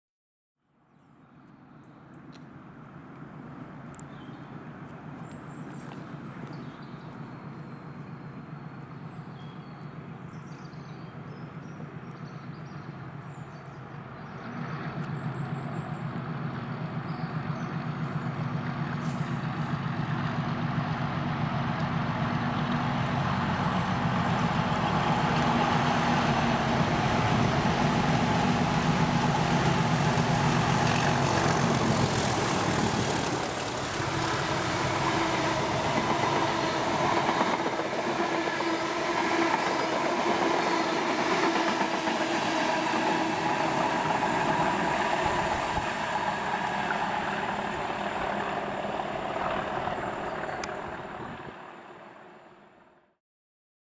(HD) DRS Large Logo 37402 & 37424 THRASH through Palmers Green with GA MK3s - 1/1/20
Here we see DRS 37402 & 37424 storming through Palmers Green with a full rake of Greater Anglia MK3s working from Bounds Green to Norwich TRSMD on the 1st of January 2020.